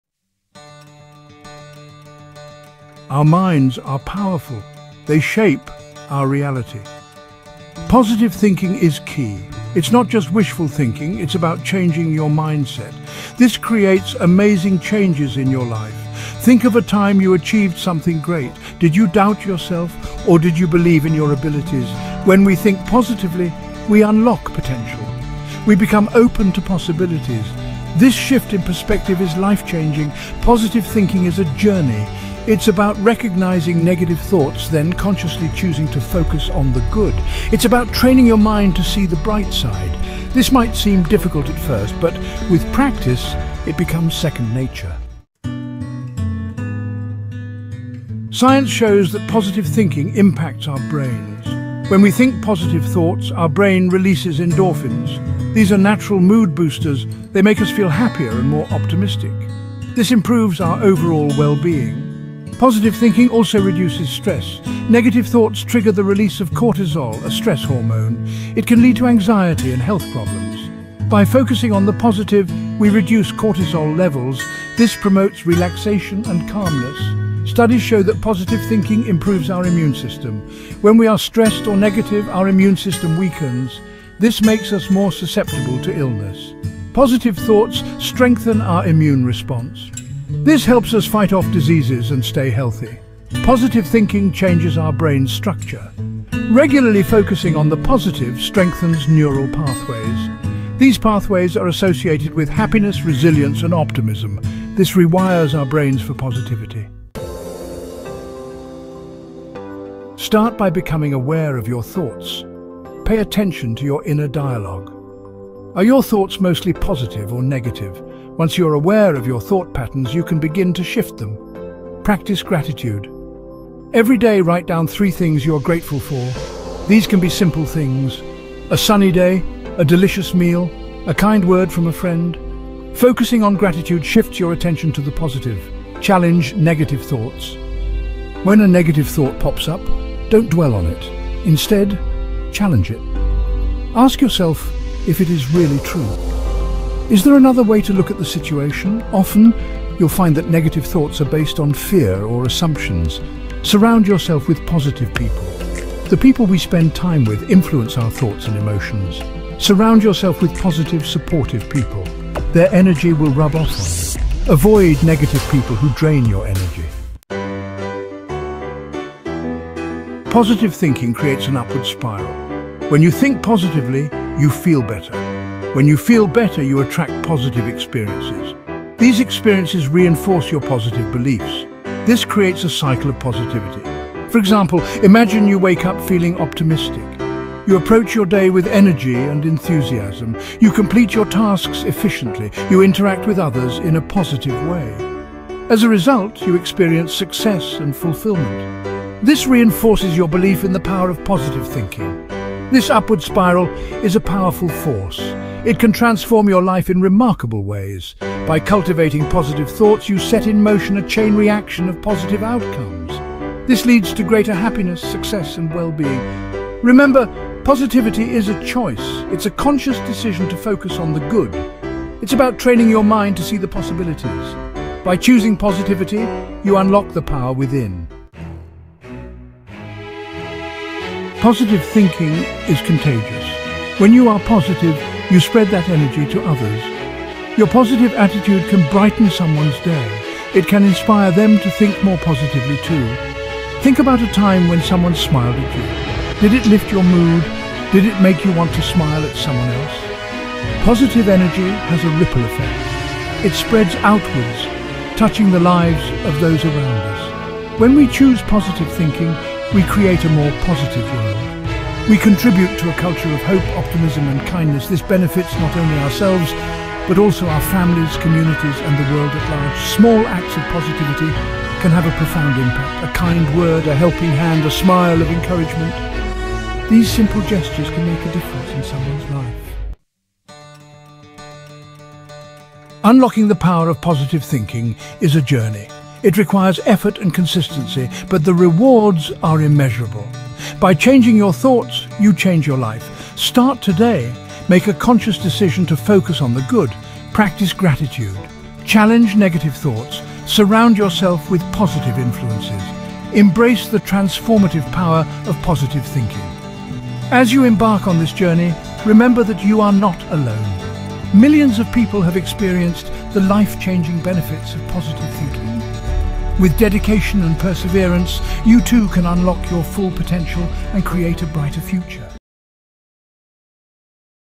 Unlocking the Power of Positive Thinking: A Brain's Journey
Unlocking the Power of Positive Thinking: A Brain's Journey" invites you on a transformative exploration into the incredible potential of positive thinking. Discover how your mindset shapes your reality and learn powerful strategies to cultivate optimism and unleash the full power of your brain. Join us on this empowering journey to embrace positivity and unlock new levels of growth and fulfillment in your life!